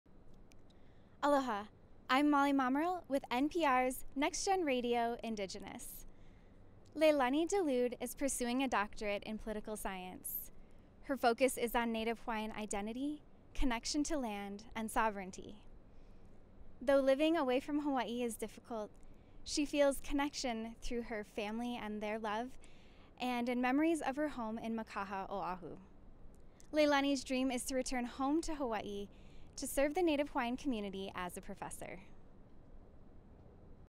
Molly Mamaril reports for NPR's Next Generation Radio
Molly Mamaril speaks with Leilani De Lude, a political science doctoral student at the University of New Mexico in Albuquerque. Her studies are focused on Native Hawaiian identity, kinship with land and sovereignty. Though living away from Hawaiʻi is difficult, De Lude maintains her connection to home through her family and memories of her childhood in Mākaha, Oʻahu. Despite the high cost of living, De Lude’s dream is to return to the Islands as a professor to serve her Native Hawaiian community.